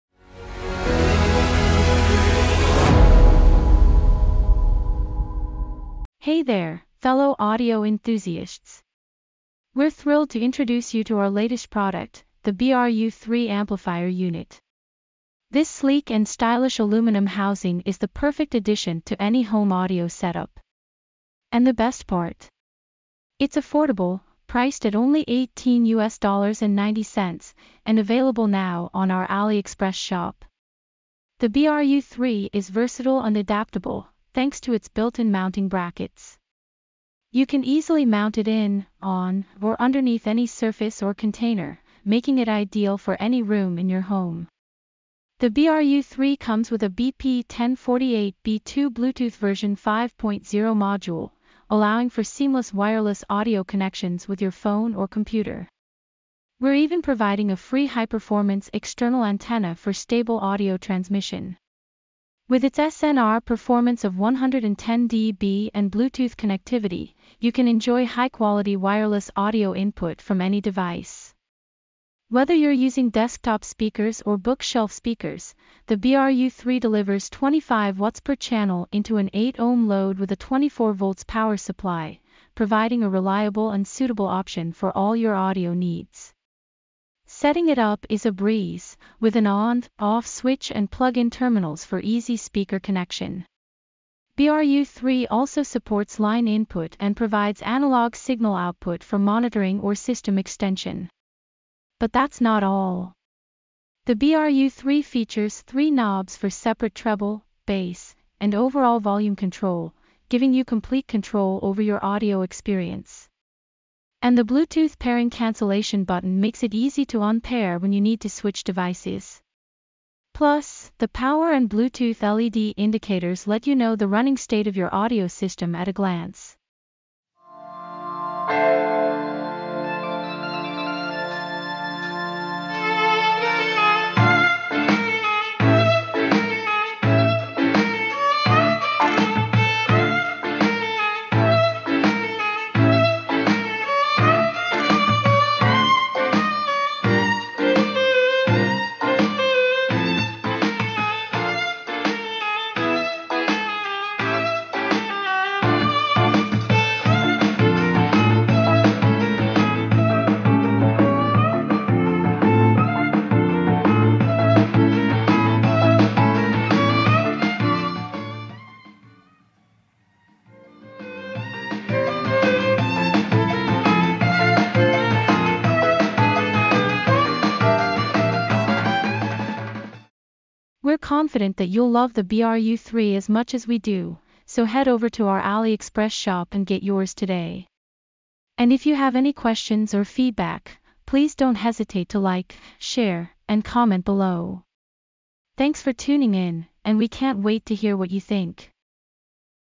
Stereo 25W Bluetooth Amplifier Unit with Preamp Output and Separate Bass, Treble, Volume Control
BRU3 is a stereo amplifier unit integrated with Bluetooth V5.0 for wireless audio connection, delivering 25W per channel into an 8Ohm load with a 24V power supply. In addition to Bluetooth input, BRU3 supports 3.5mm line input. It also provides preamp output for monitoring or system extension. Three knobs are mounted on BRU3 for separate treble, bass and volume control. Thanks to its built-in mounting brackets. You can easily mount it in, on, or underneath any surface or container, making it ideal for any room in your home. Low output power, wireless audio connection and plug-in terminals make BRU3 suitable for desktop audio applications, bookshelf speaker, home audio and most audio DIY projects.